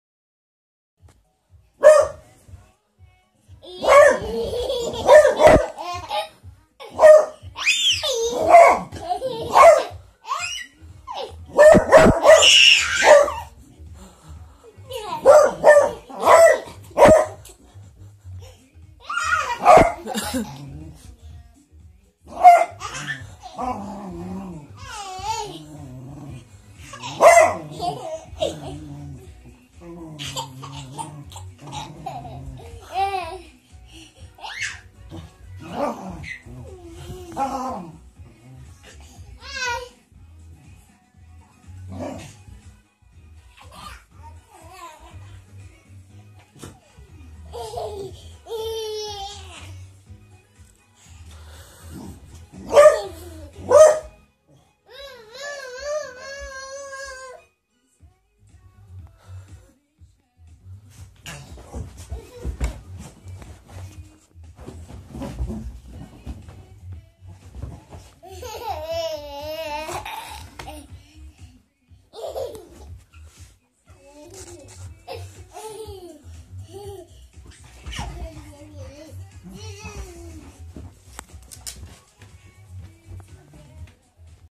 Baby and my Beagle Kira
Stelios vs Kira.. Całe moje życie z moimi kochanymi wariatami.